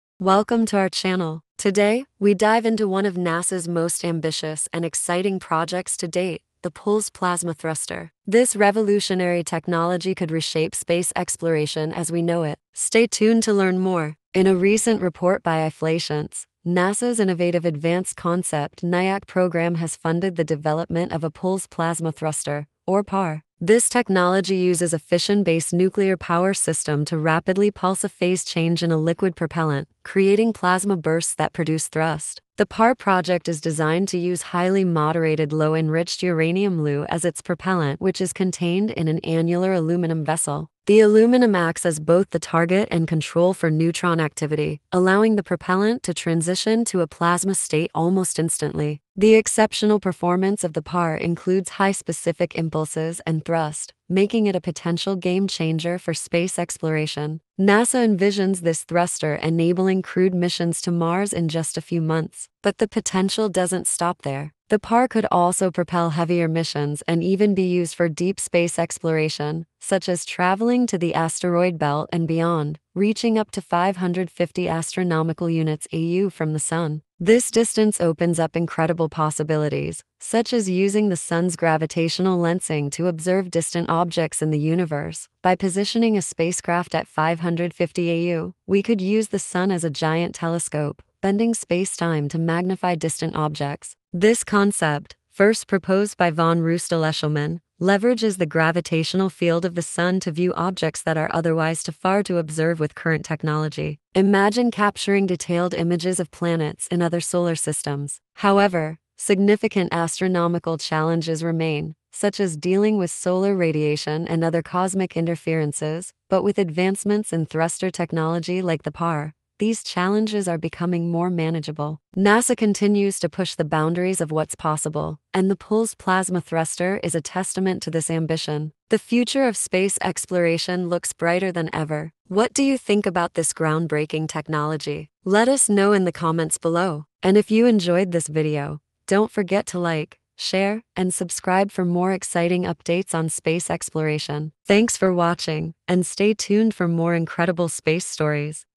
NASA's Pulsed Plasma Thruster: The Future of Space Exploration!
Discover NASA's groundbreaking Pulsed Plasma Thruster (PPR) in this exciting video! Funded by the Innovative Advanced Concept (NIAC) program, the PPR promises to revolutionize space exploration with its fission-based nuclear power system. Learn how this cutting-edge technology could enable crewed missions to Mars in months and explore the potential for deep space missions reaching up to 550 astronomical units (AU) from the Sun.

Key highlights:
- High specific impulses and thrust capabilities of the PPR.
- Potential for Mars missions and beyond.
- Using the Sun's gravitational lensing to observe distant objects in the universe.

Watch as we delve into the details of this ambitious project and explore the future of space travel!

📢 What are your thoughts on the Pulsed Plasma Thruster?